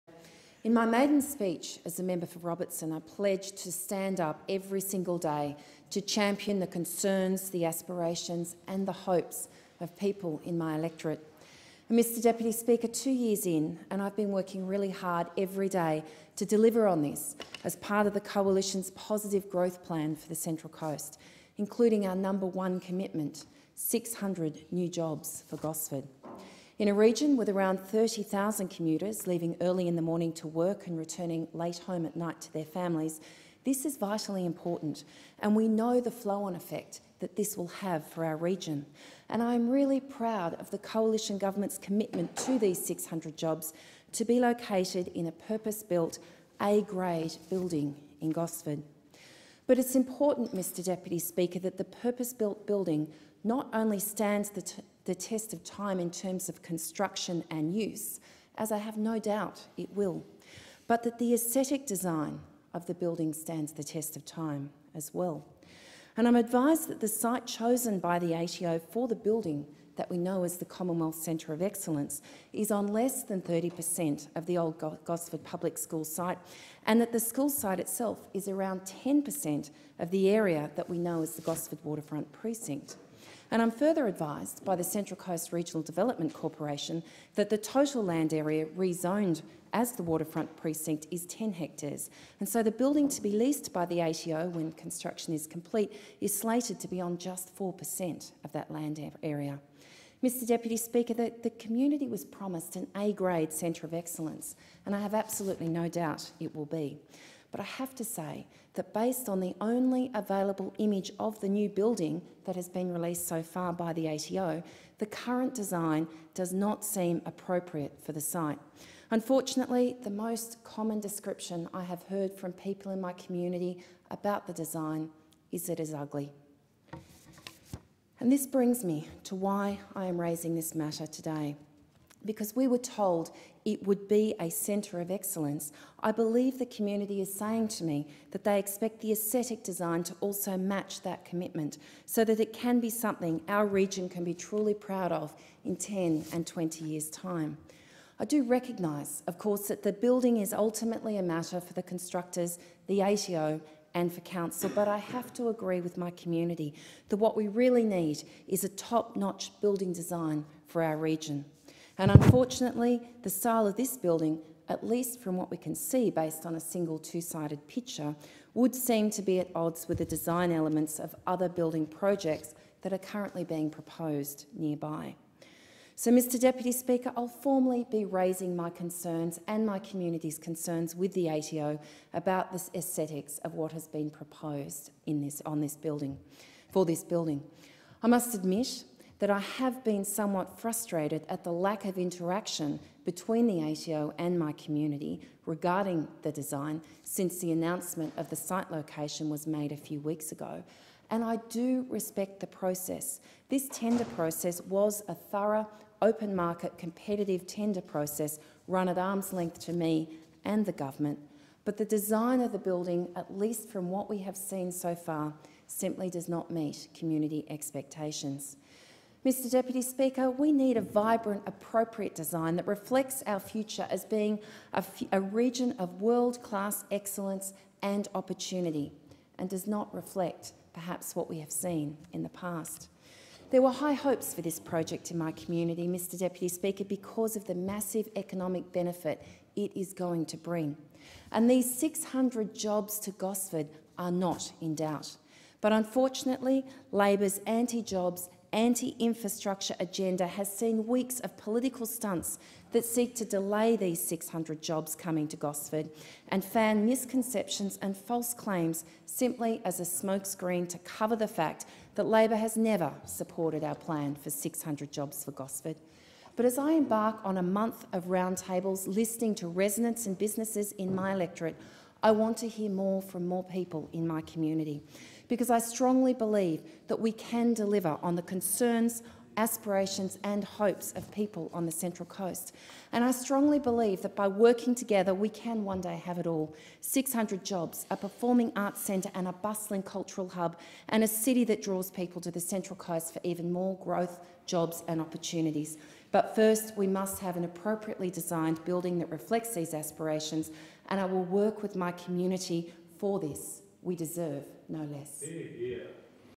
Gosford deserves the right Centre of Excellence
Federal Member for Robertson Lucy Wicks has stood up in Parliament to express community concerns about the Commonwealth Agency Centre of Excellence coming to Gosford, with 600 jobs.
"There were high hopes for this project because of the massive economic benefit it would bring.
And these 600 jobs for Gosford are not in doubt.
But I have to say that based on the only available image of the new building that has been released so far by the ATO, the current design does not seem appropriate for the site. Unfortunately the most common description that I hear from people about the design is it is ugly."